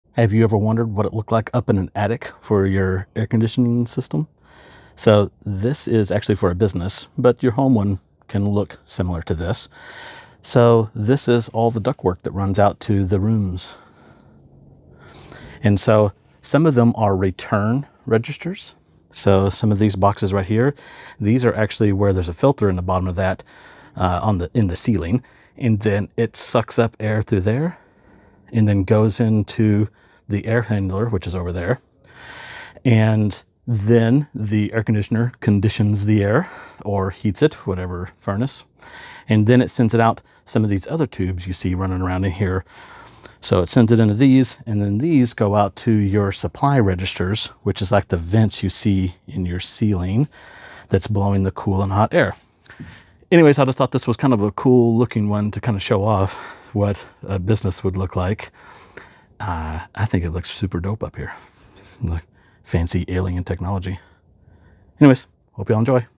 Here’s a behind-the-scenes look at the heart of a building's comfort: the HVAC system in the attic.
This web of ductwork is essential for your air conditioning. The larger boxes are return registers, which pull air through filters. That air then travels to the air handler, where it's cooled or heated. Finally, the conditioned air is distributed back into the rooms through the supply ducts and out your vents.

Now you know what’s happening above your head to keep you comfortable!

⸺ We service the following areas ⸺⸺⸺⸺⸺⸺⸺⸺